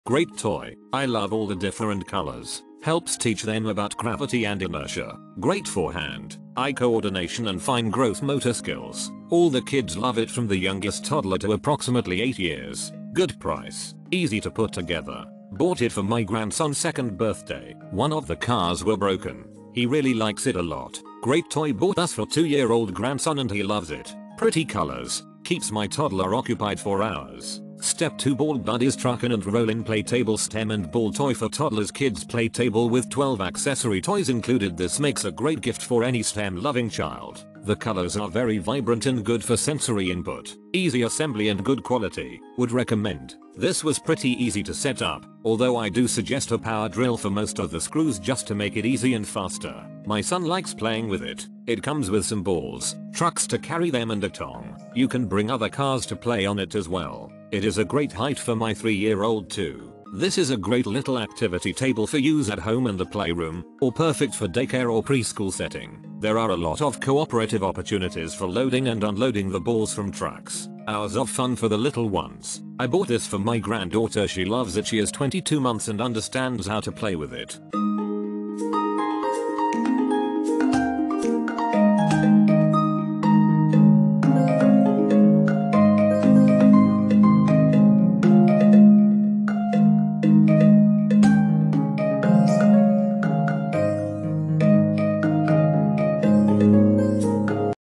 Step2 Ball Buddies Truckin' & Rollin' Play Table | STEM & Ball Toy for Toddlers | review
Main Features:
by entering your model number.. Get the fun rollin’ with the ball Buddies kids’ indoor activity table, ideal for multiple toddlers and preschoolers to play together!. Up and down, and around the bend! Load up a ball Buddies dump truck with the hinged ball drop tower or central ball track, then take them around the track and through the tunnels.. Use the ball-picker tongs to carefully set play balls on the trucks, or onto the track and watch them roll.. Watch out for the clicking gates! Little ones have so many ways to play taking their ball Buddies trucks around the toddler activity table to drop off and pick up cargo.. In the box: activity table base, 2 ball Buddies trucks, ball-picker tongs, 10 play balls.